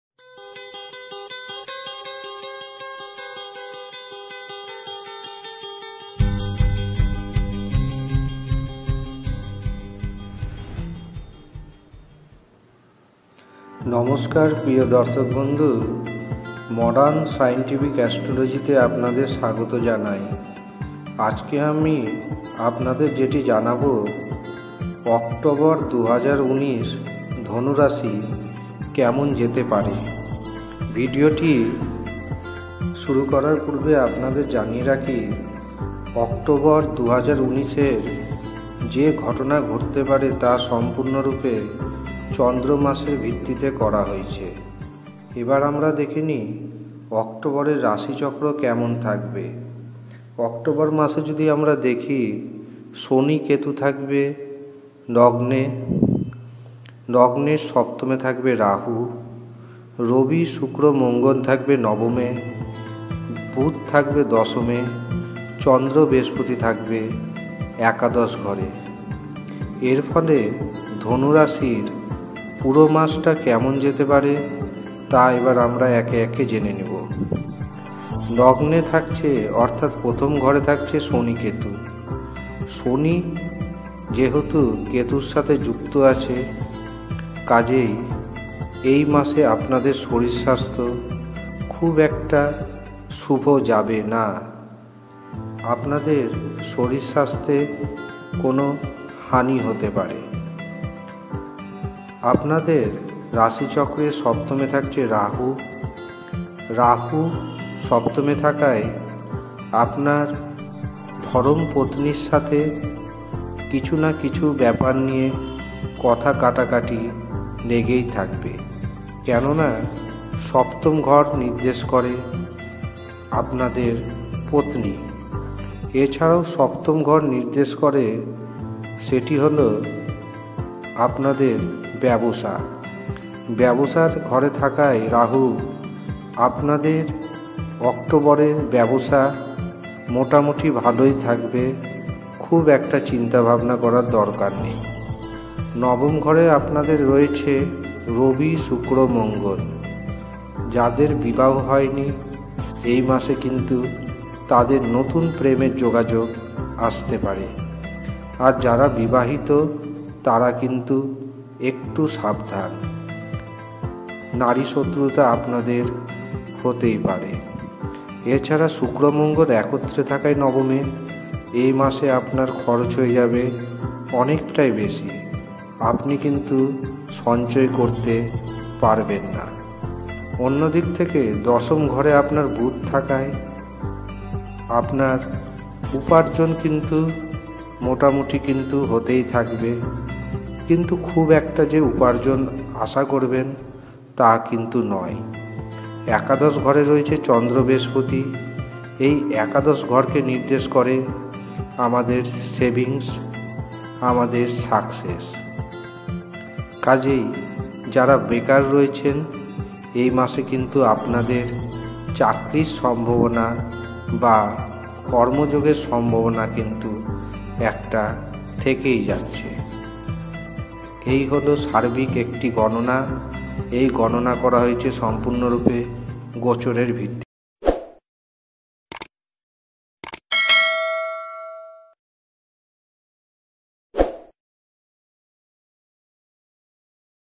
ধনু রাশি অক্টোবর 2019। দুর্গাপূজার মাসে ধনু রাশির বিপদ । ধনুরাশিঅক্টোবর2019 DhonuRasiOctober2019.
ধনু রাশি 2019 অক্টোবর  মাস কেমন যাবে তা জানতে ভিডিওটি মনোযোগের সাথে সম্পূর্ণ দেখতে থাকুন আপনাদের যদি কিছু জিজ্ঞাসা থাকে অবশ্যই আমাকে কমেন্ট বক্সে কমেন্ট করে জানাতে পারেন সঙ্গে থাকুন লিখতে থাকুন নিত্য নতুন ভিডিও ধন্যবাদ।